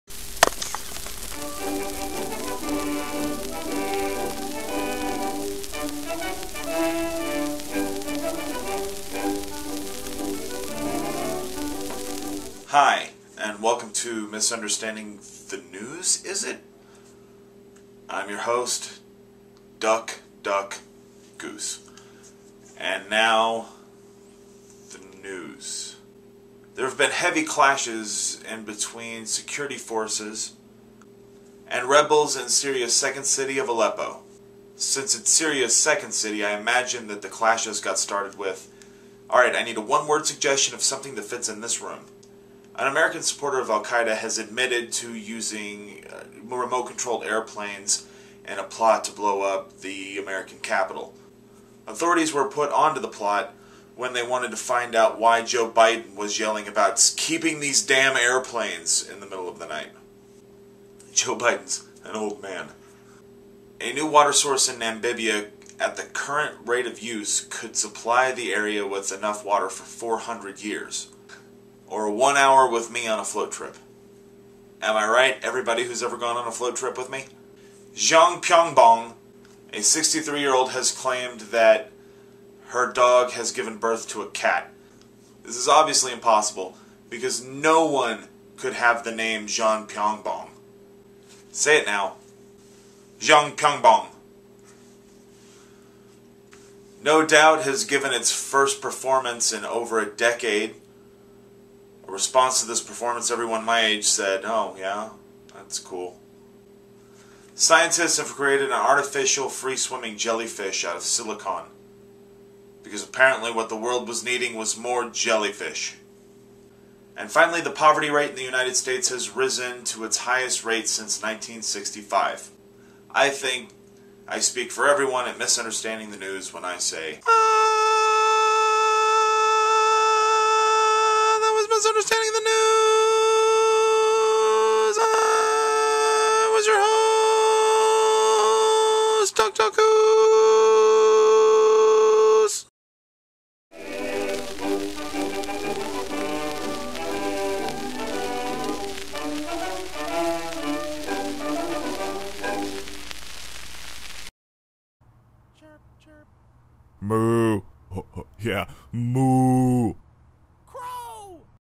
Misunderstanding the News July 23rd, 2012
Stand-up comedian Doug Driesel Jr. brings you the news of the last few days. Or, at least, as he best understands it.

Today, Doug discusses Syrian clashes, remote controlled plane bombings, Namibia water sources, a dog giving birth to a kitten, No Doubt's first performance in over a decade, artificial jellyfish and the poverty rate in America.